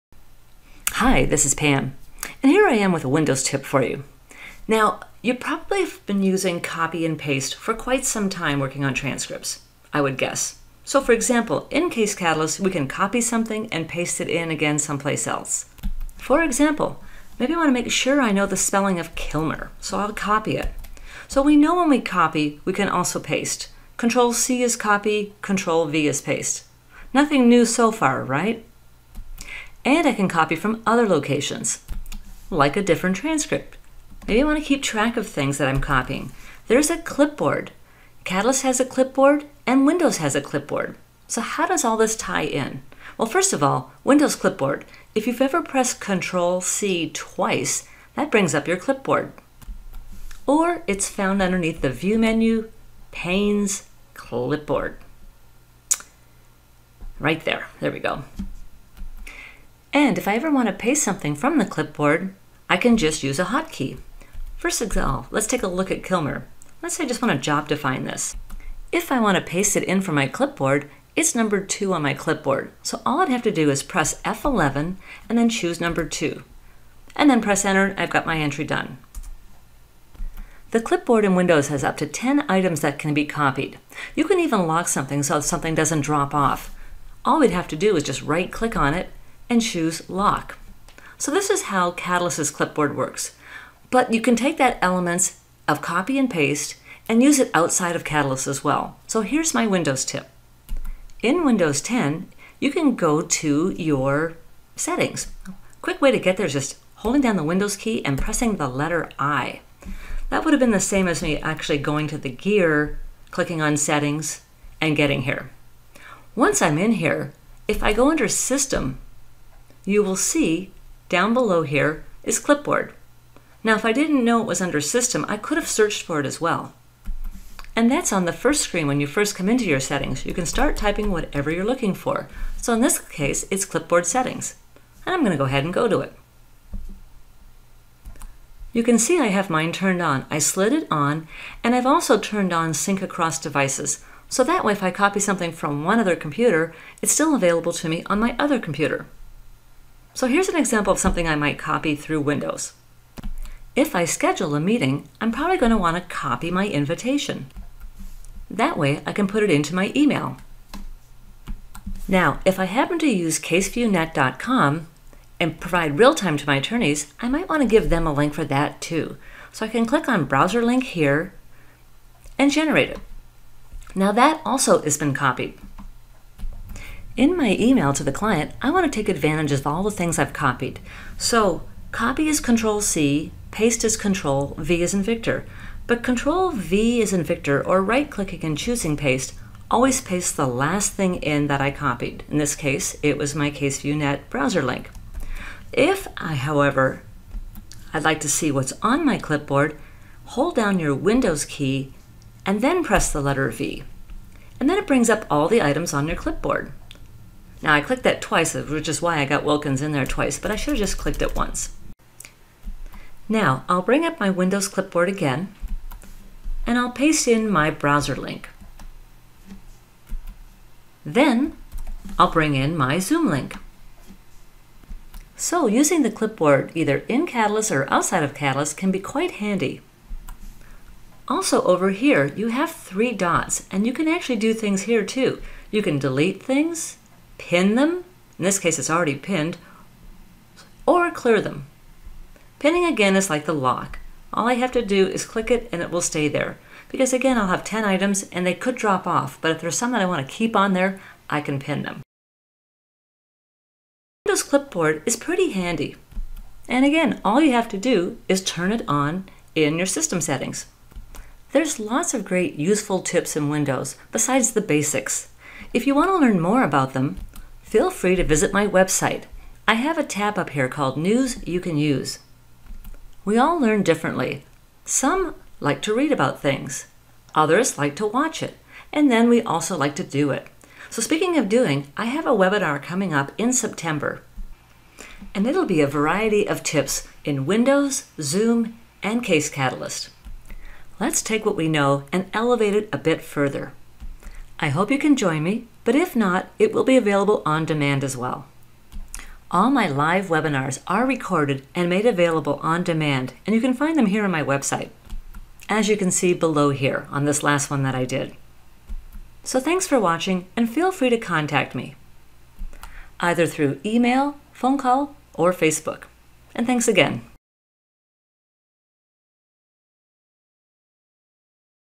Windows Clipboard
Keep track of what's on your clipboard whether in Catalyst or Windows.  Here's how to do it.